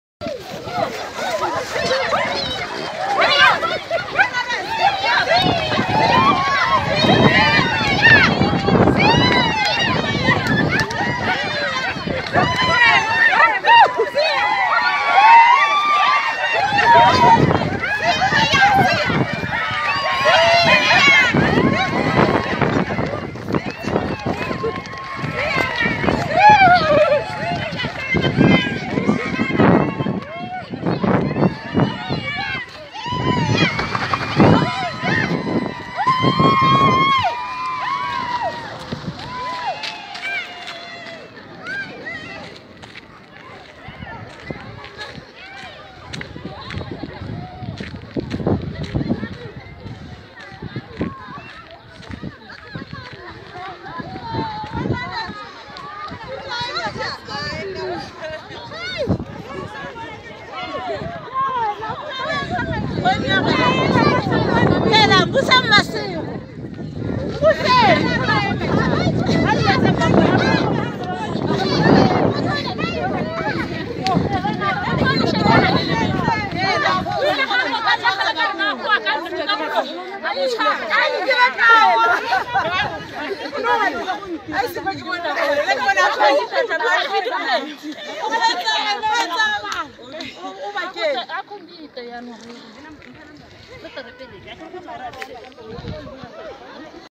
What a culture 🤔 Ke fo kae teng fa? 😳🙄😭😂🤣😲🤓😮😱 Ngwetsi 🙋‍♀️ just runs away and Basadibagolo...
What a culture 🤔 Ke fo kae teng fa? 😳🙄😭😂🤣😲🤓😮😱

Ngwetsi 🙋‍♀️ just runs away and Basadibagolo have to catch her if they fail the pay 💰 for her to return so that lenyalo le tswelele

👌👌👌Good morning my people... It is a NEW DAWN ❤